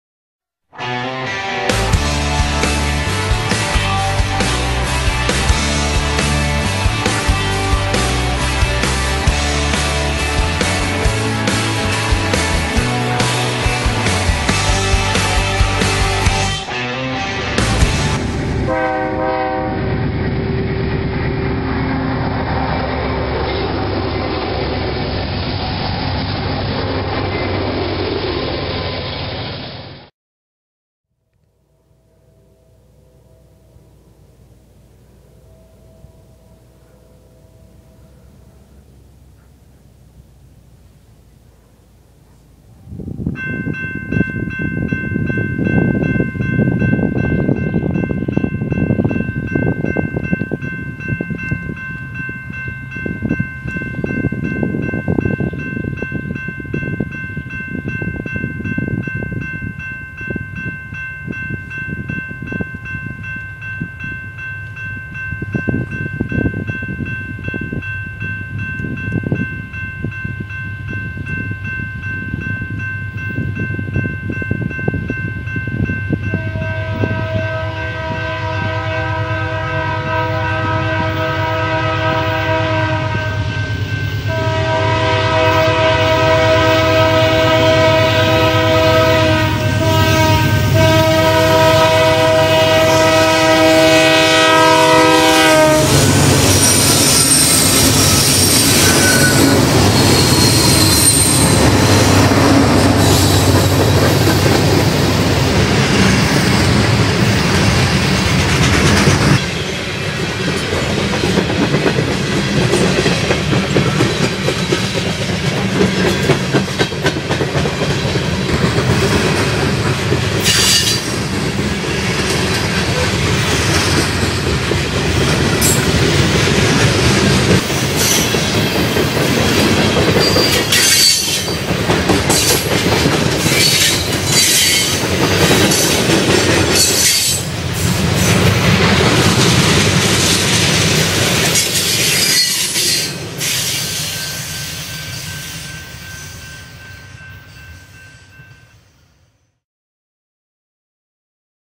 HiDef: BNSF 4578 South - Colorful Lash Up!
Well I heard that there was a Southbound coming so I ventured to a different location. We are set up at the County Line Road Grade Crossing and the Laurel to Denver Manifest tops the grade at Berthoud Hill as they cross into Boulder County from Larimer.